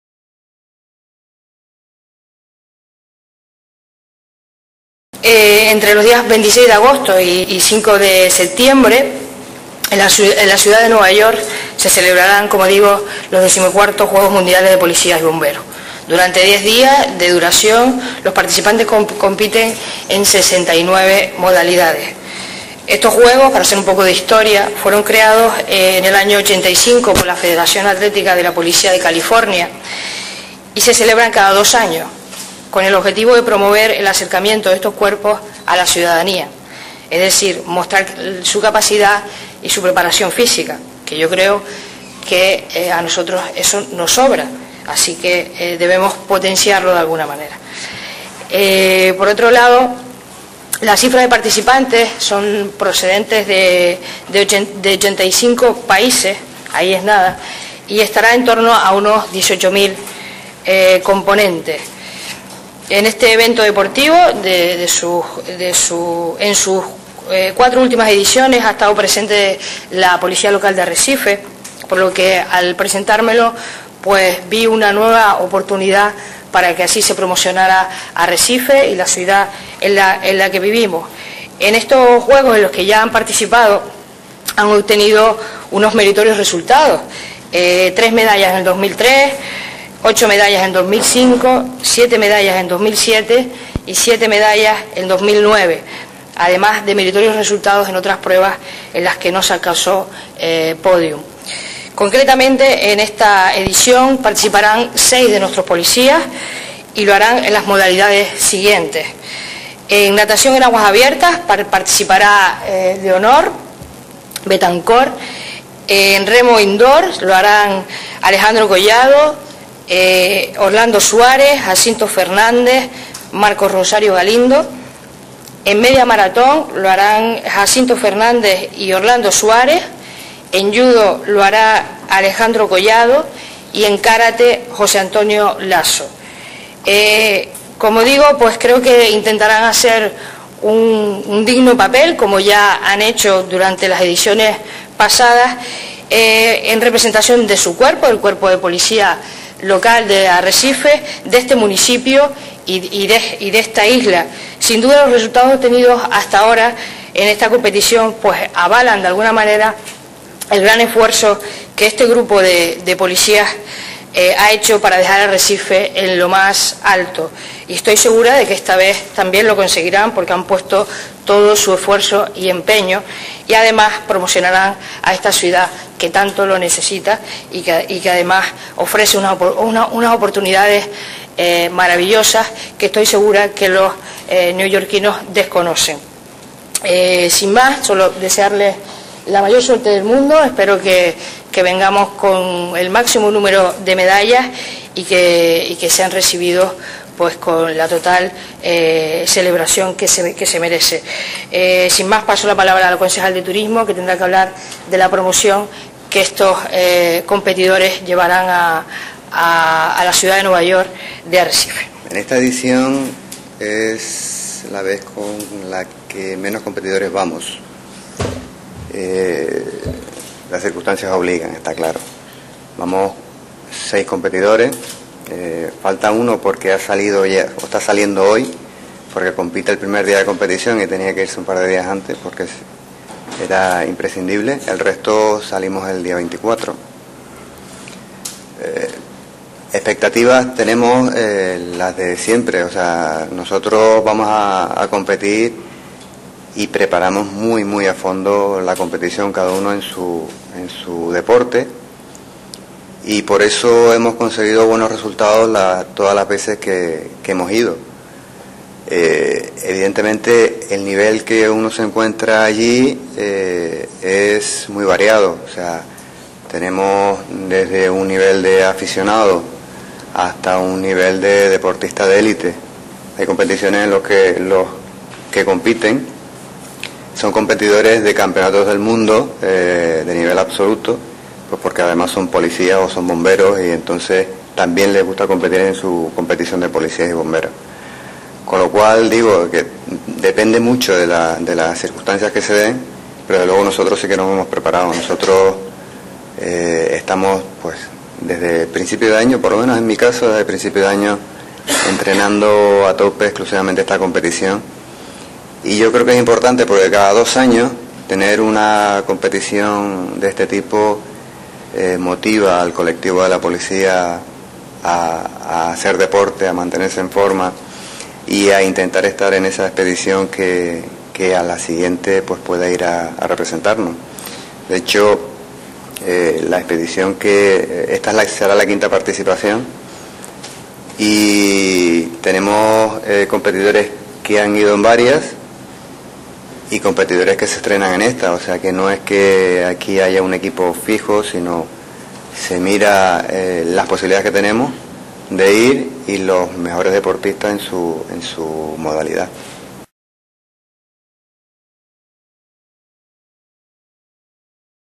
Agentes de Arrecife participarán en los Juegos Mundiales de policías y bomberos
Agentes de la Policía Local de Arrecife participarán en los XIV Juegos Mundiales de Policías y Bomberos, que se celebrarán entre los días 26 de agosto y 5 de septiembre en la ciudad de Nueva York.

DiariodeLanzarote.com
[Martes, 23 de agosto de 2011] [07.00]

Se trata de una competición atlética donde toman parte más de 15.000 participantes, profesionales de los cuerpos de Policías y Bomberos de todo el mundo de 70 países, y compiten en 69 modalidades deportivas.

En las cuatro últimas ediciones ha estado presente una representación de la Policía Local de Arrecife, habiéndose obtenido unos meritorios resultados (tres medallas en 2003, ocho medallas en 2005, siete medallas en 2007 y siete medallas en 2009, además de destacados puestos en aquellas pruebas en las que no se alcanzó el pódium).

En la que será la quinta participación consecutiva de los agentes de Arrecife, competirán en las modalidades de natación en aguas abiertas, remo indoor, media maratón, judo y karate.

Los seis policías locales que se trasladarán a Nueva York son: Leonor Betancort, Alejando Collado, Orlando Suárez, Jacinto Fernández, Marcos Rosario Galindo y José Antonio Lasso.

Los Juegos Mundiales de Policías y Bomberos fueron creados en el año 1985 por la Federación Atlética de la Policía de California, y se celebran cada dos años, con el objetivo de promover el acercamiento de estos cuerpos a la ciudadanía.

Cabe destacar que Leonor Betancort competirá en natación en aguas abiertas, mientras que Alejando Collado, Orlando Suárez, Jacinto Fernández, y Marcos Rosario Galindo lo harán en remo indoor.

Además, Jacinto Fernández y Orlando Suárez también participarán en la media maratón, Alejandro Collado en Judo y José Antonio Lasso en Kárate.

Todos ellos se mostraron esta mañana muy entusiasmados con su nueva participación en estos Juegos Mundiales de Policías y Bomberos, "que nos ayudan a estar en forma y a hacer deporte", y que también se han celebrado en Canadá, Australia, Suecia, o España, entre otros países.